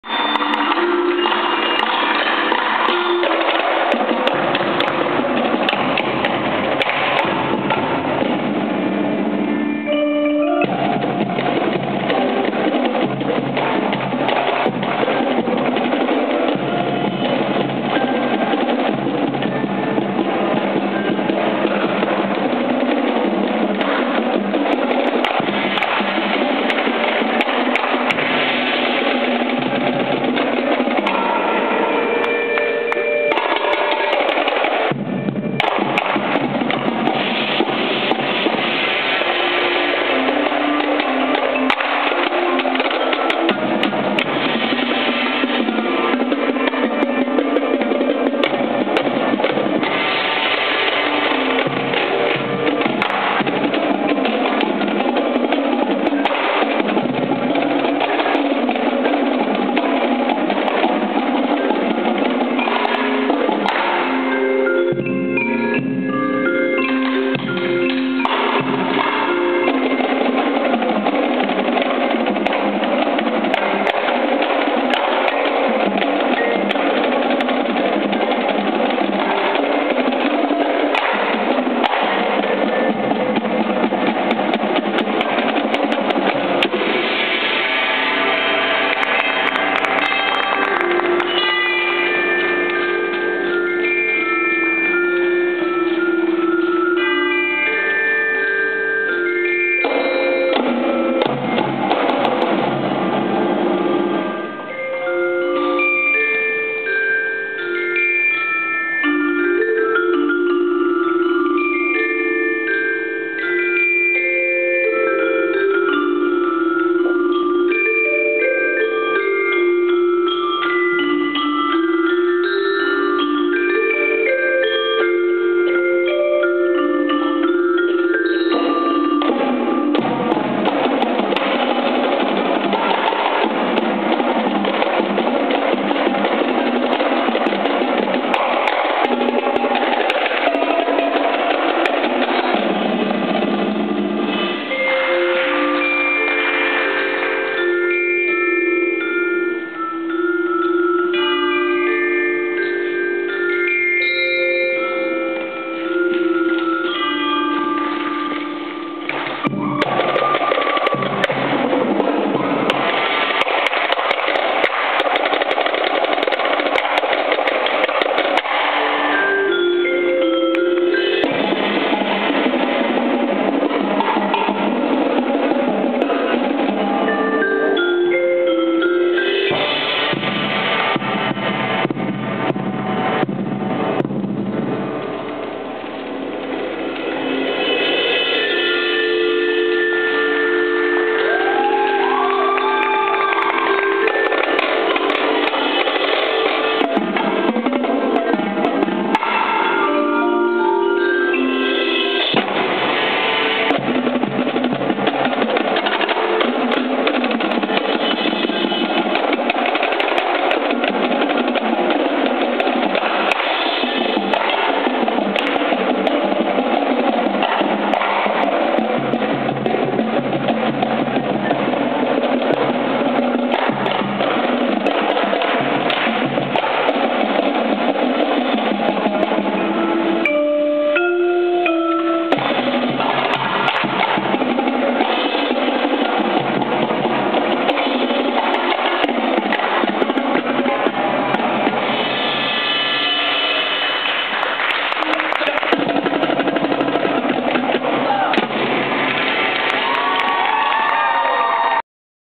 Patrick Henry High School Winter Drum Line Home Show 2012
This is Patrick Henry High School Winter Drum Line's home show for the 2012 winter season. Show is titled, "Things That go Bump in the Night." Missing the opening solo and a little bit of beginning in this video.